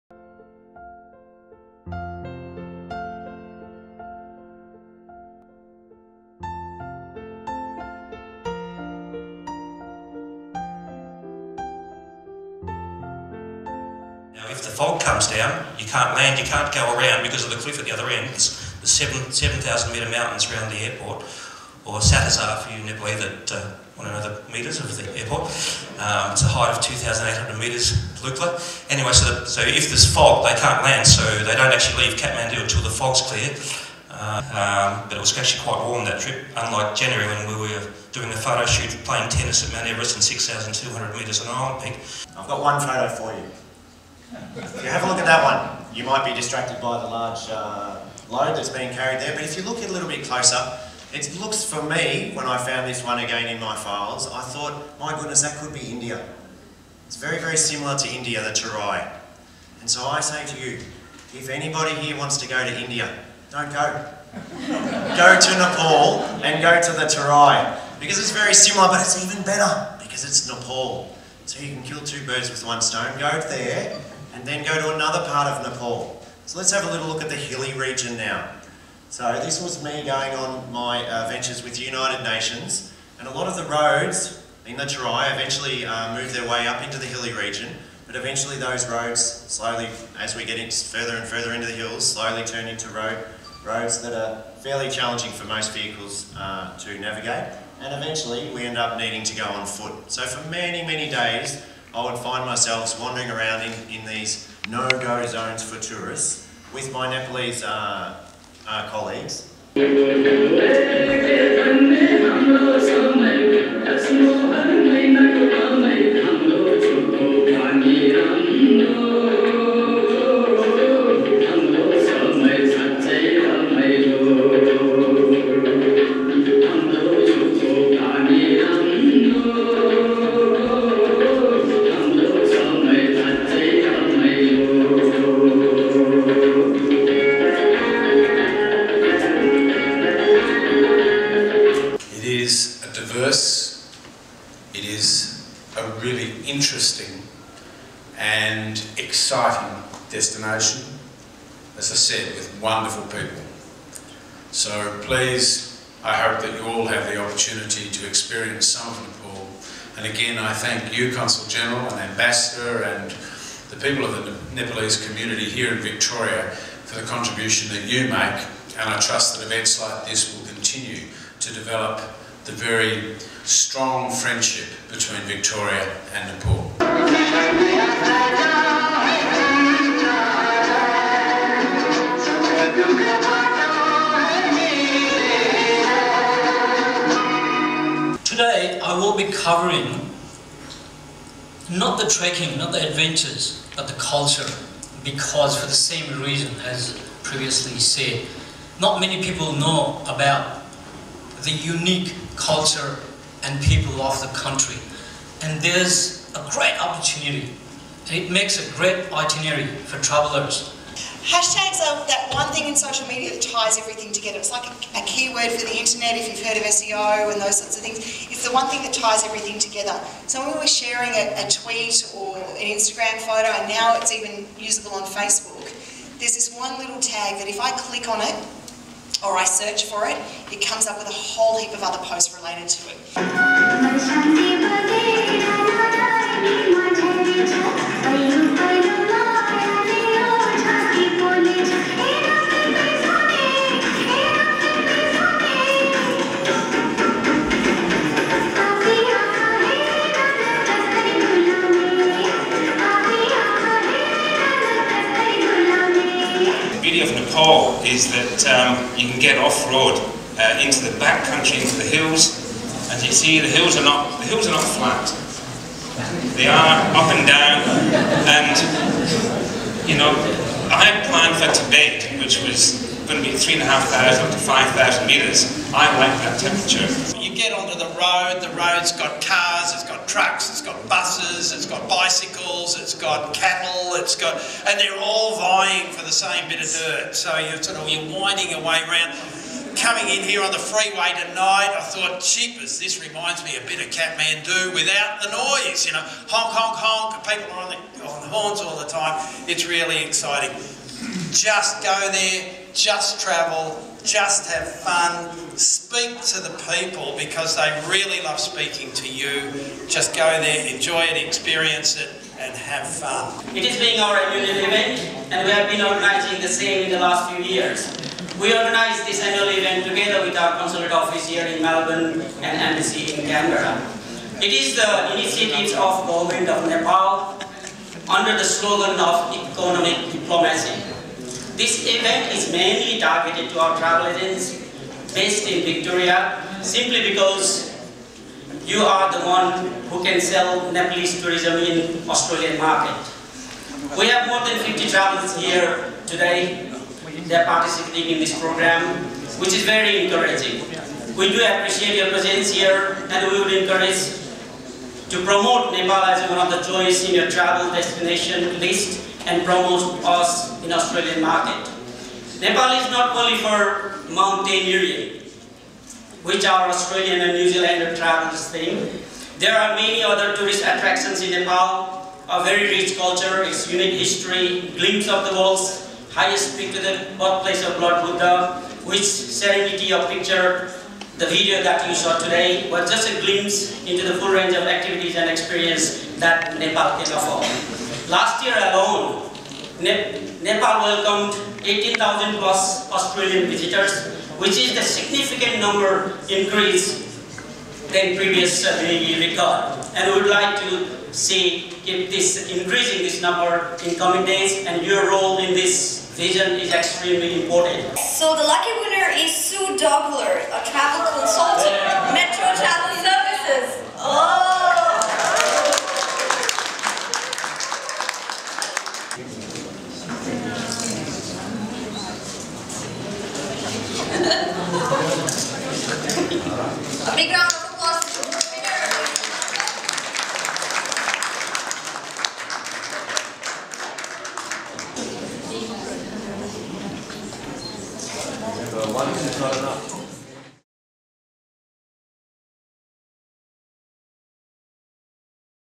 Nepal Promotion Forum 2014
The Consulate of Nepal in Victoria, in association with the Embassy of Nepal, Canberra organised "Nepal Tourism Promotion Cocktail Reception" on 26 of June 2014.